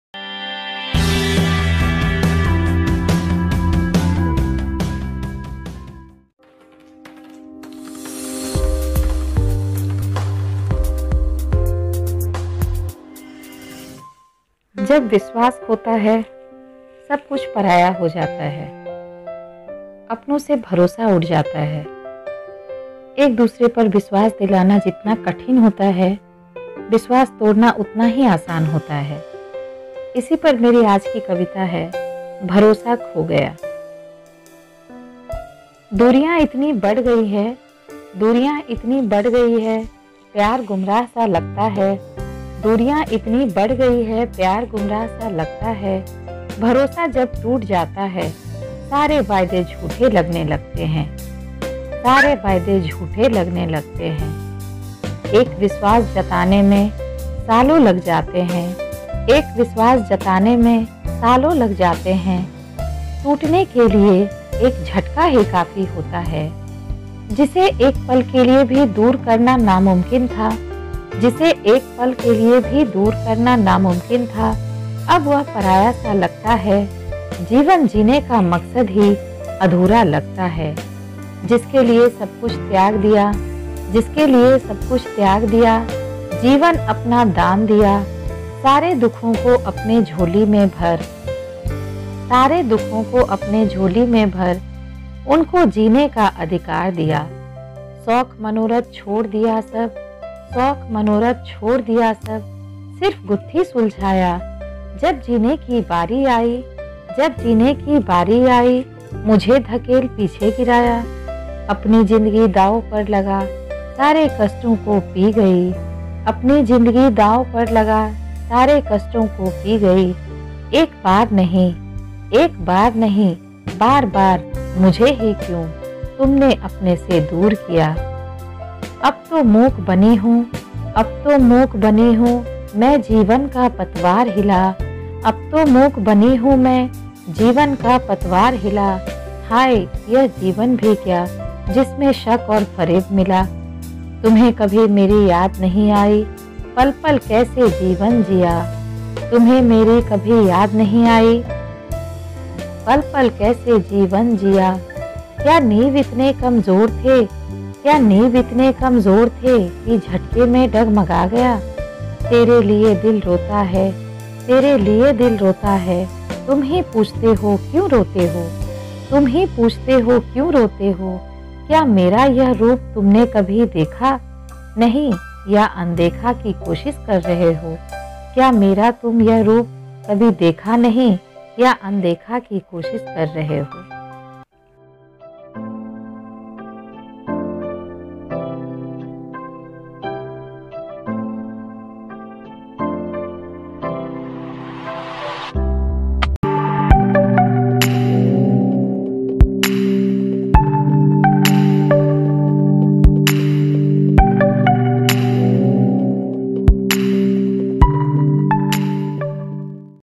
भरोसा खो गया ।।। Lost the trust...
a poem on the condition when trust is lost...

Self composed poem ..
Composer - Saraswati Jha
Voice- Saraswati jha
No part of this video can be used for any commercial purpose without prior permission

For some poem topic suggestions and to follow our every post get in touch with our social handles